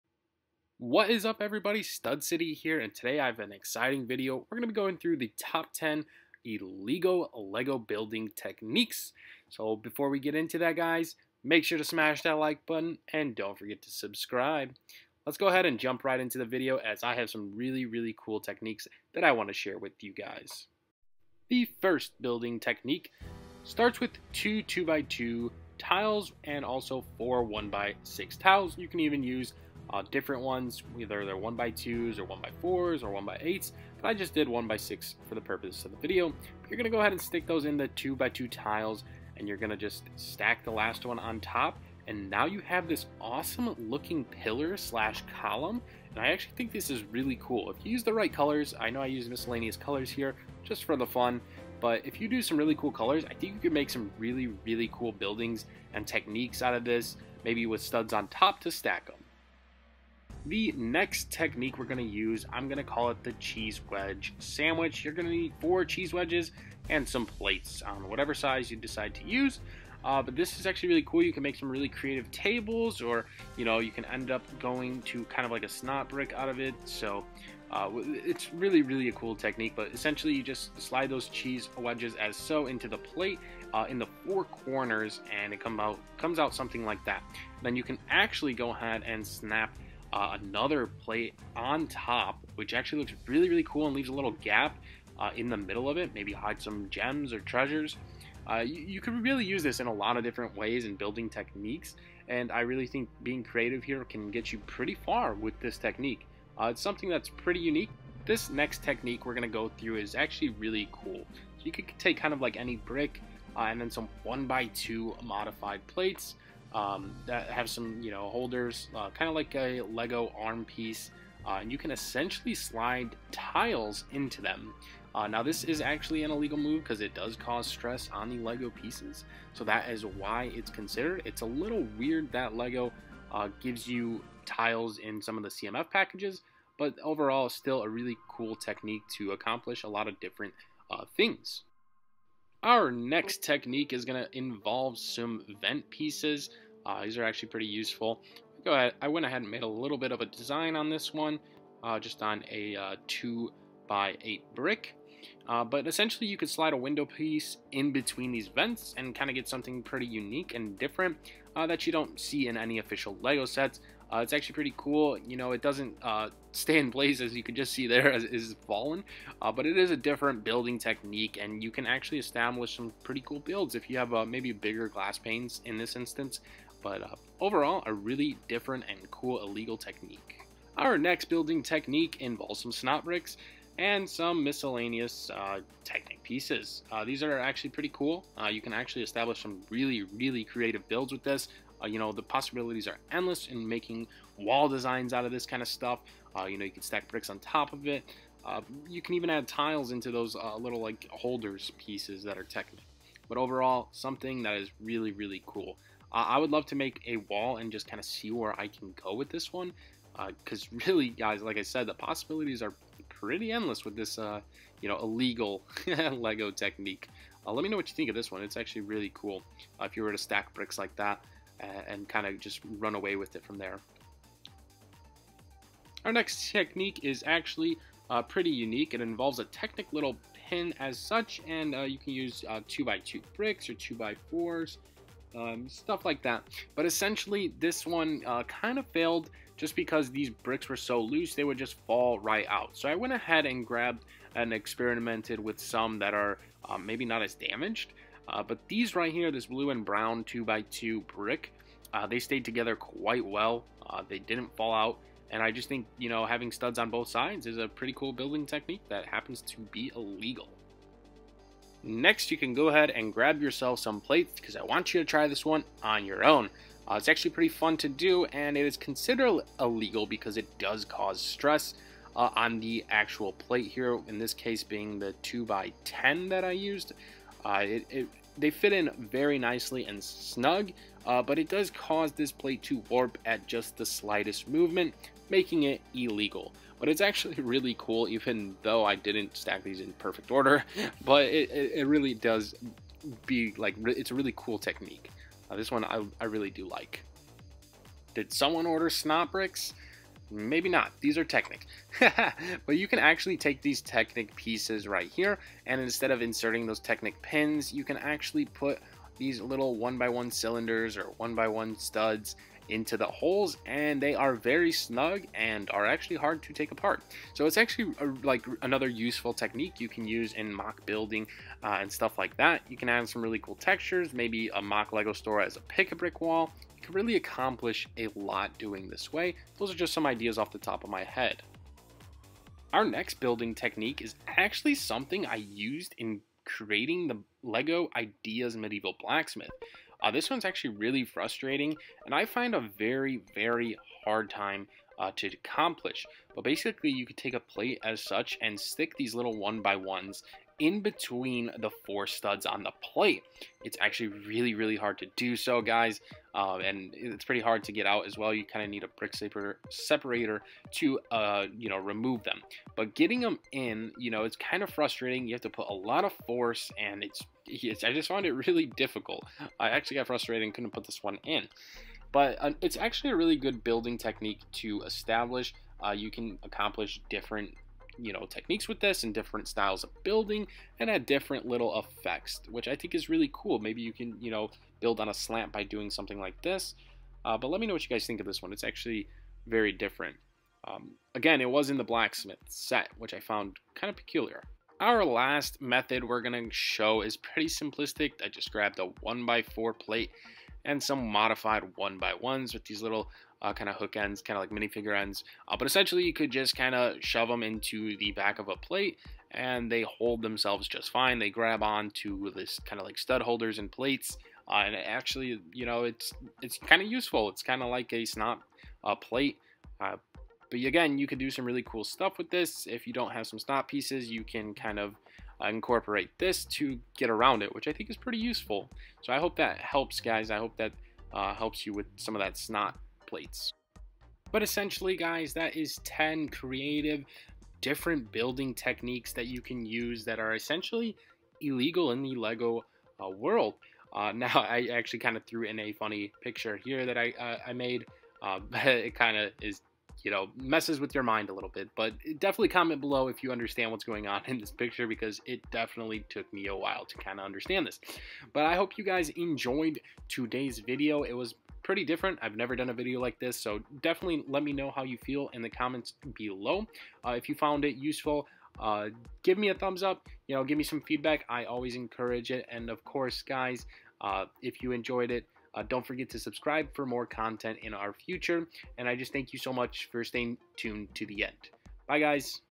TOP 10 ILLEGAL LEGO BUILDING TECHNIQUES | LEGO TIPS AND TRICKS
10 Illegal LEGO building techniques that you might have never seen.

Leave feedback on if you enjoyed this video as I hope you enjoyed the LEGO tips and tricks!